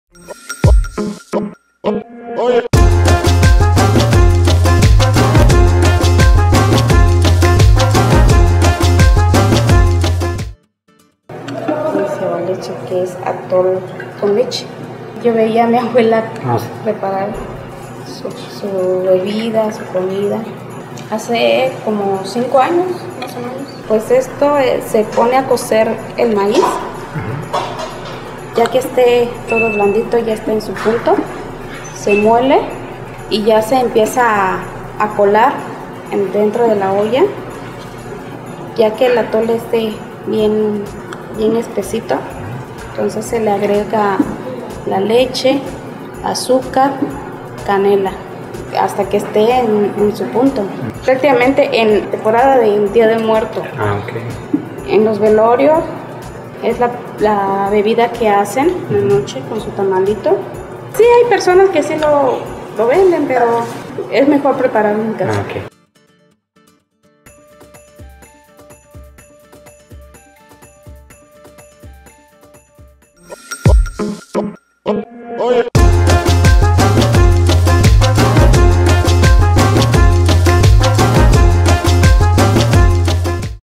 Preparacion de "Ni Xiaba Nidxi o Atole con Leche"
Rosa Elba Antonio Ortiz, es originaria de Asunción Ixtaltepec que pertenece al Istmo y ella nos muestra un poco sobre la elaboración del Ni Xiaba Nidxi.  Para conocer la información completa de esta bebida.

Da click en siguiente link y escucha nuestro podcast donde conocerás más información sobre las bebidas de Oaxaca.